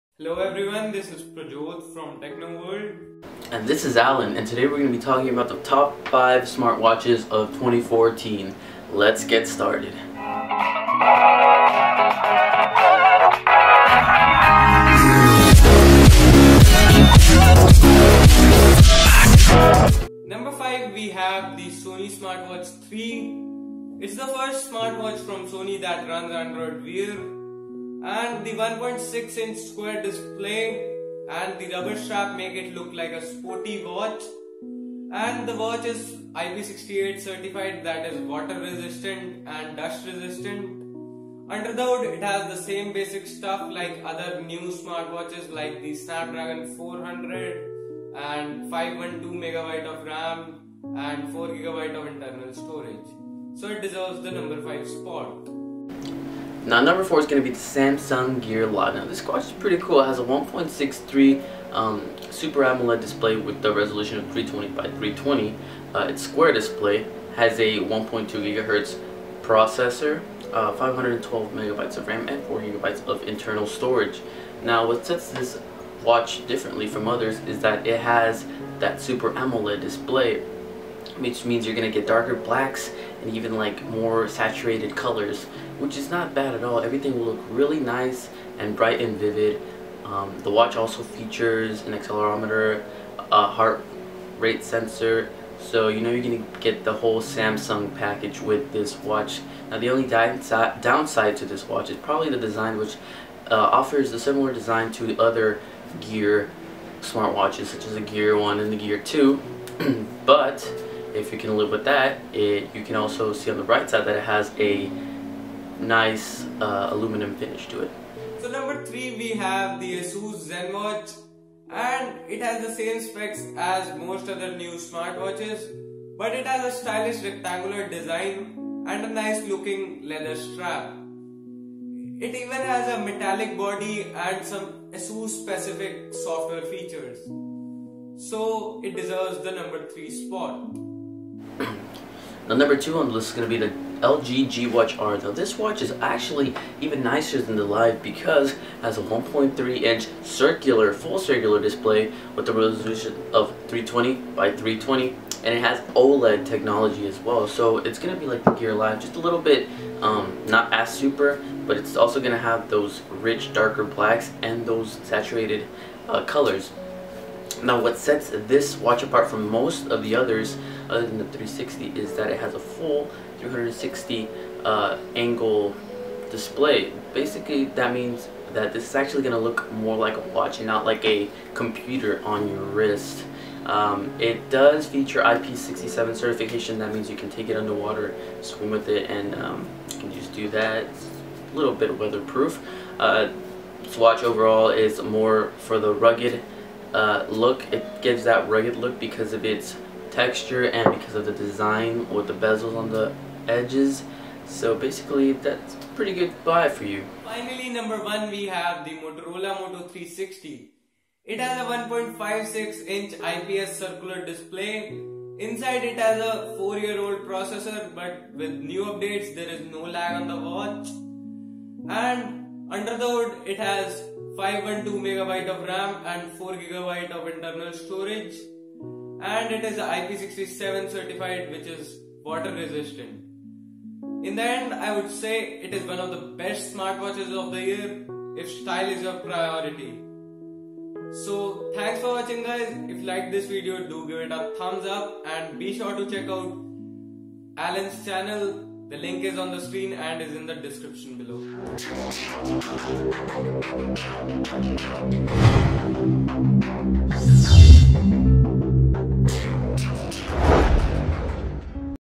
Top 5 Smartwatches of 2014 Collab!
In this video, I get along with VeryCoolAlan and we tell you about our top 5 smartwatches of 2014.